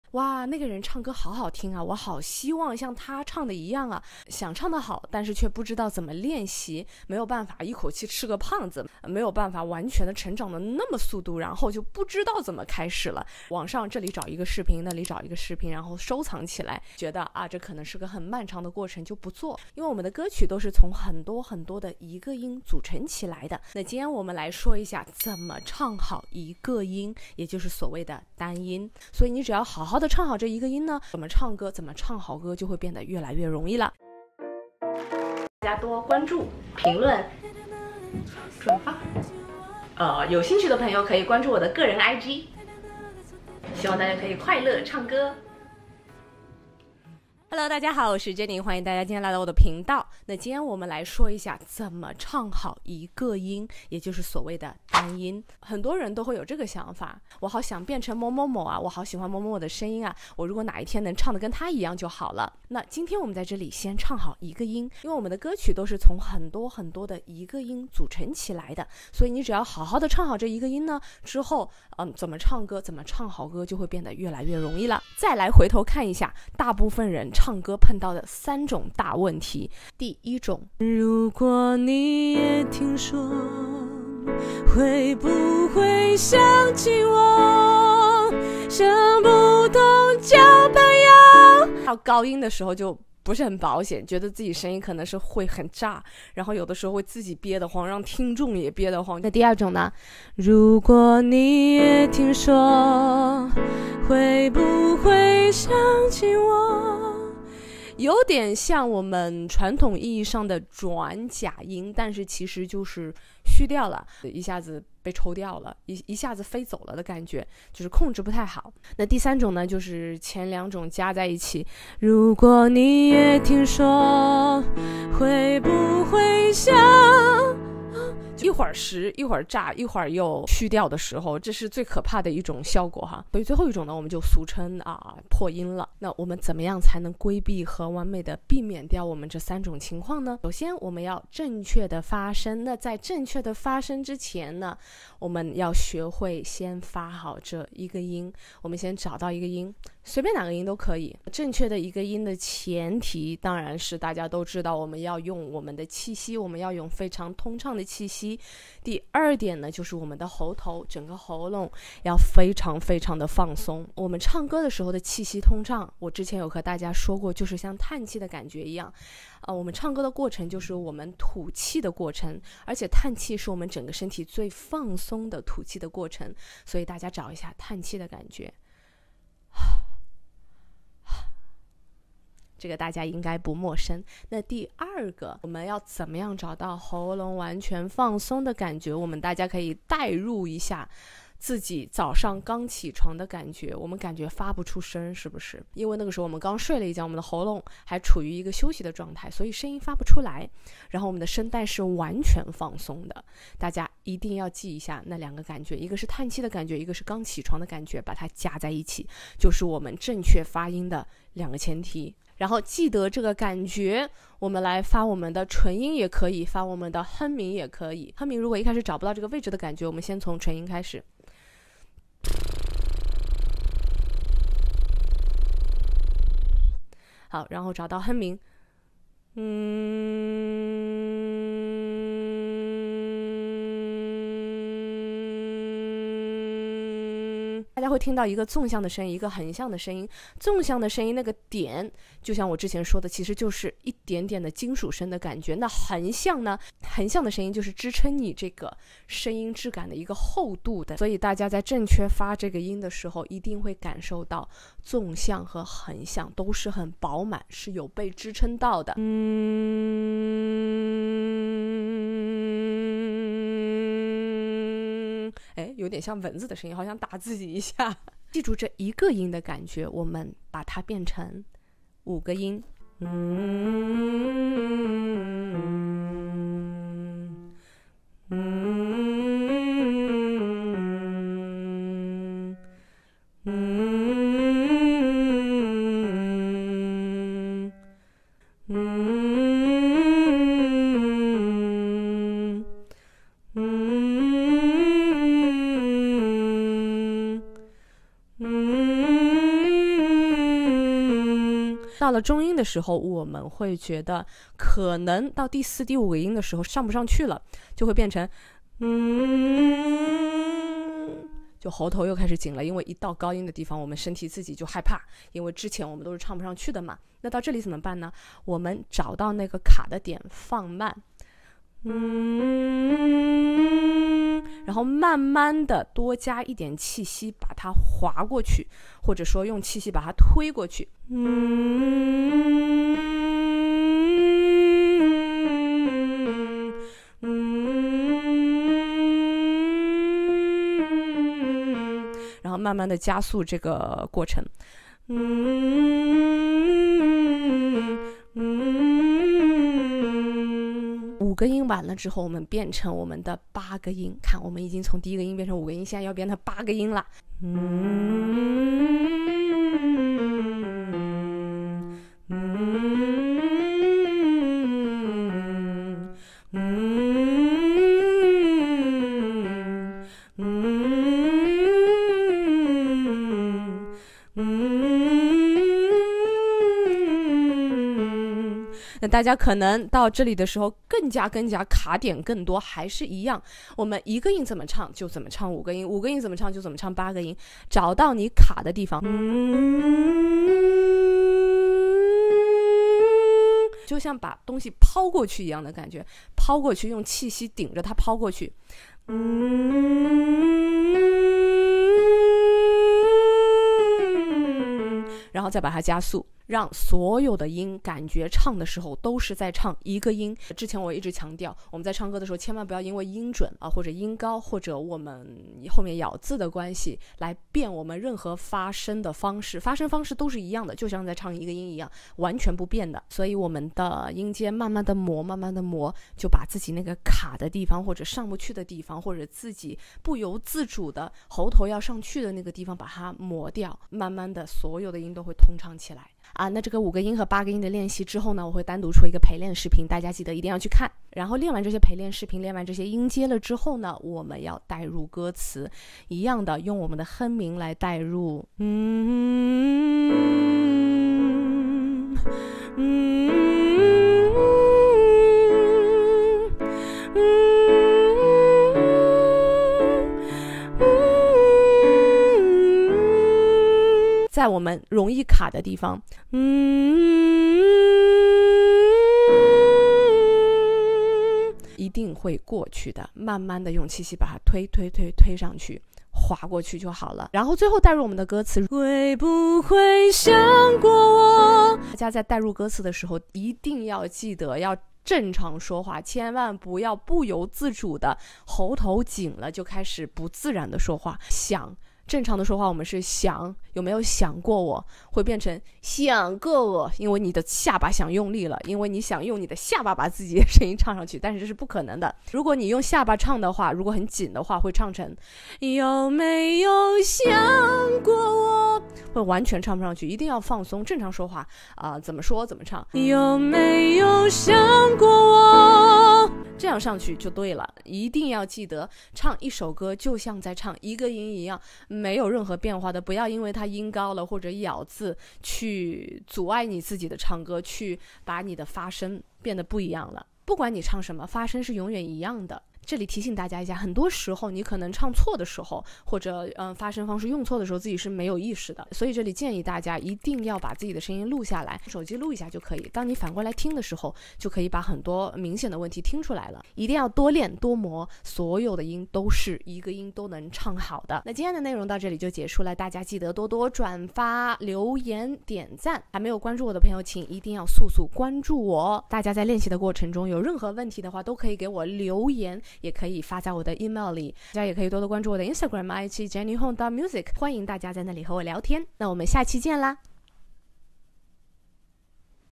正确唱好一個音！唱歌技巧最強啟蒙攻略
嗨大家好！
這次穿插一個課程，唱好一個音確實不簡單，畢竟有時候就毀在一個音了對吧！結合陪練，這個課程臉好了，唱歌突飛猛進哦！

加油吧！